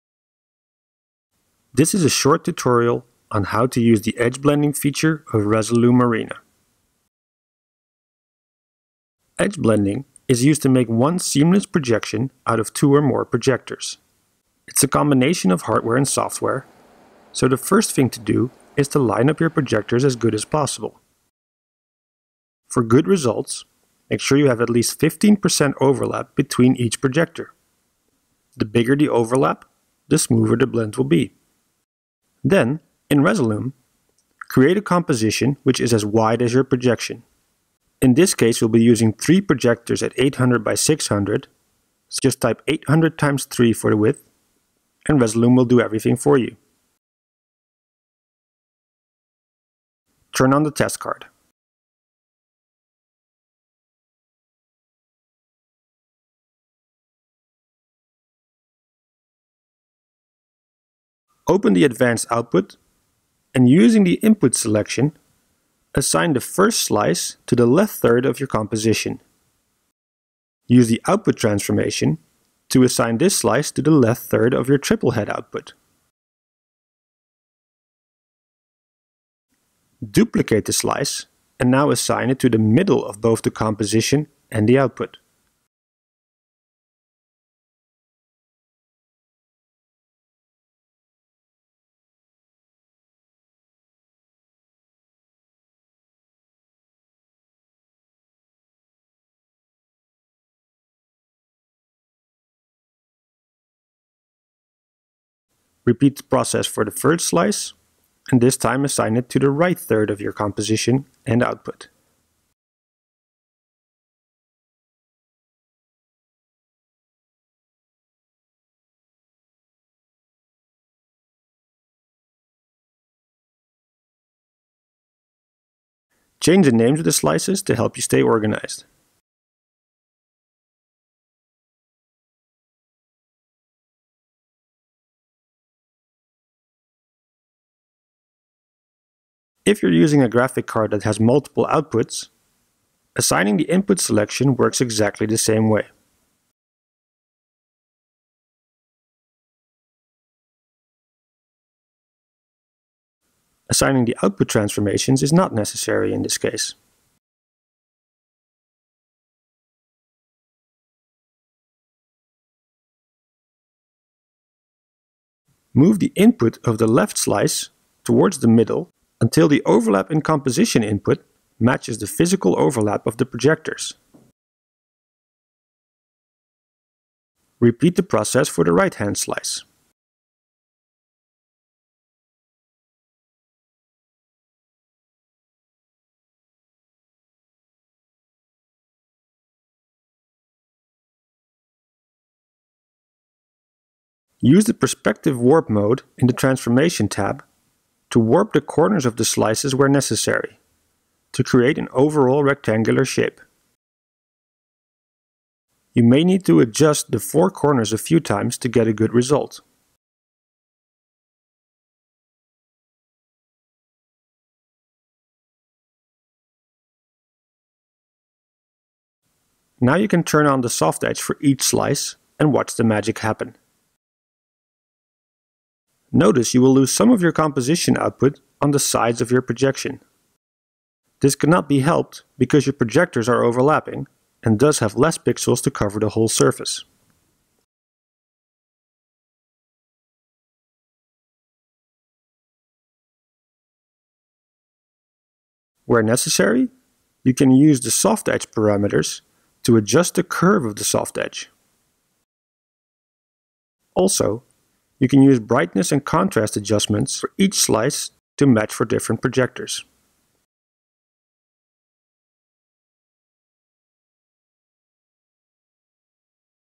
The Black Art of Edge Blending with Resolume Arena 4
A short tutorial to get you started with the soft-edging feature of Resolume Arena 4.